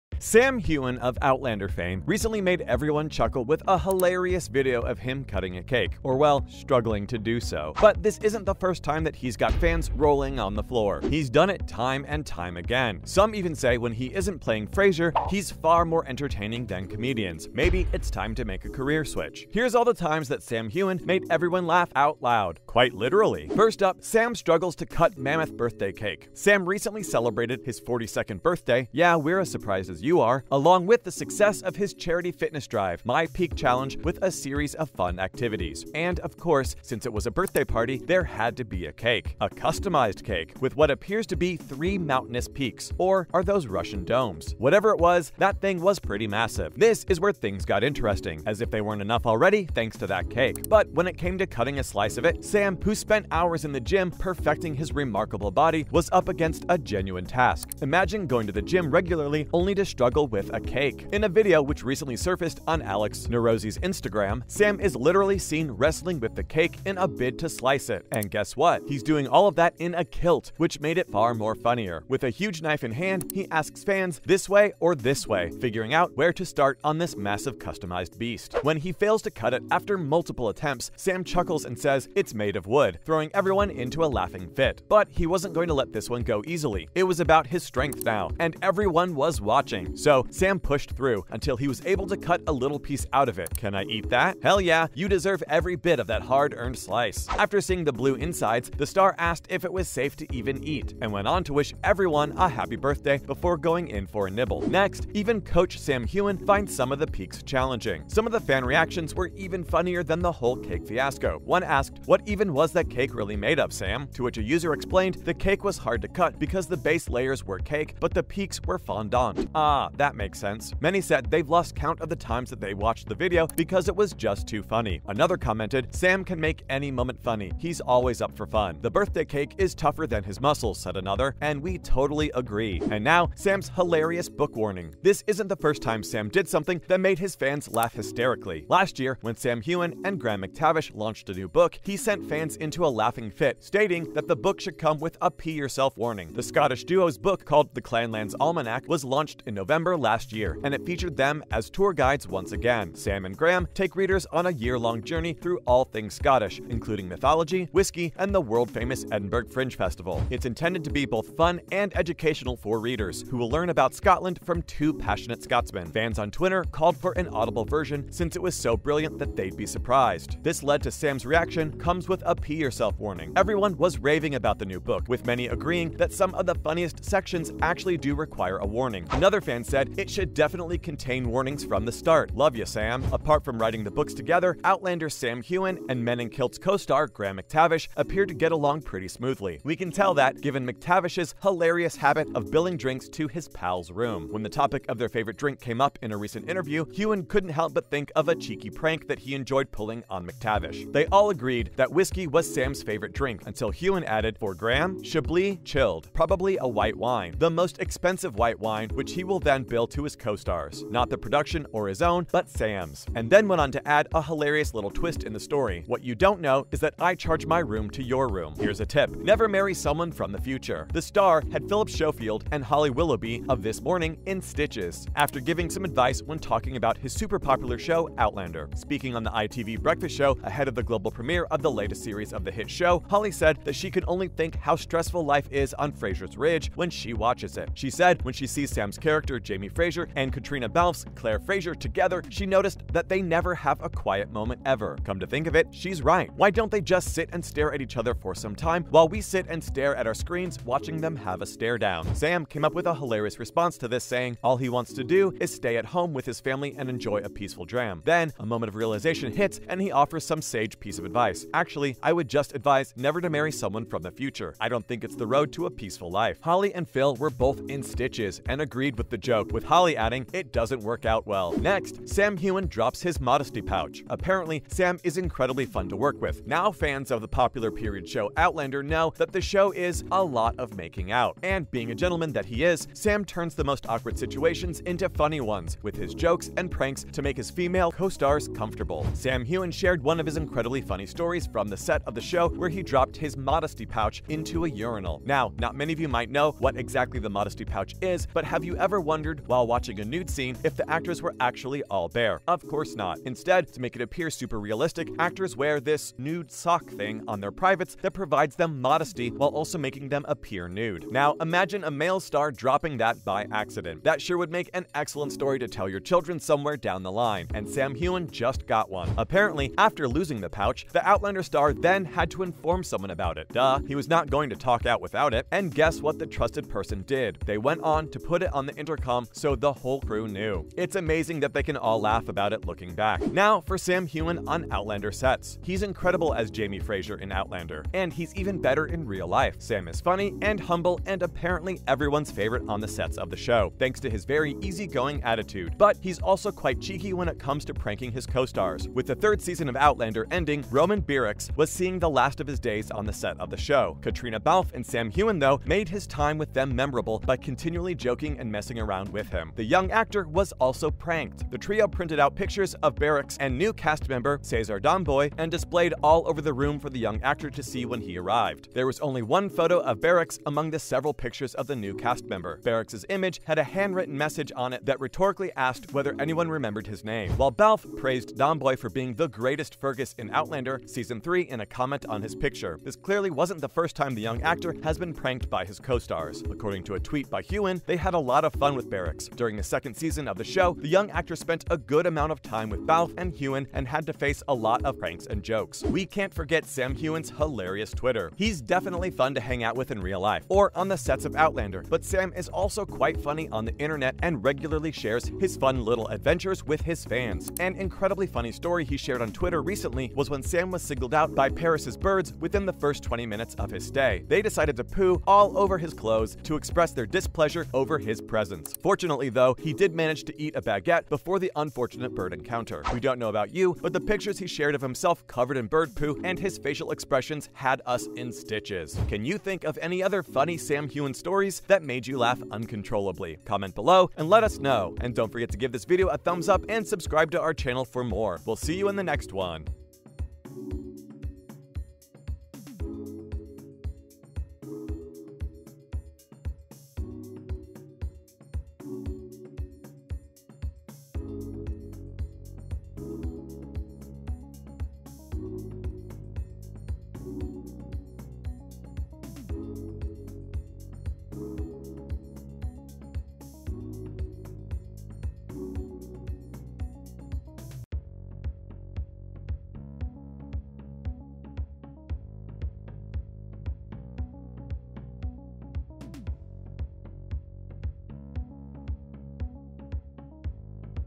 Why Sam Heughan Recently Has EVERYONE In Stitches..
Welcome back to Show Focus. On this channel right now,  we will be revealing Why Sam Heughan got everyone in stitches. Outlander star Sam Heughan just celebrated his 42nd birthday last April 30.

Sam Heughan was presented with a special cake at the My Peak Challenge Gala. And to everyone’s surprise, despite the very impressive physique of Mr. Jamie Fraser, he was kind of having a real challenge slicing the cake. Stick around on this channel, and watch this video till the end. As we are going to reveal Why Sam Heughan got everyone in stitches.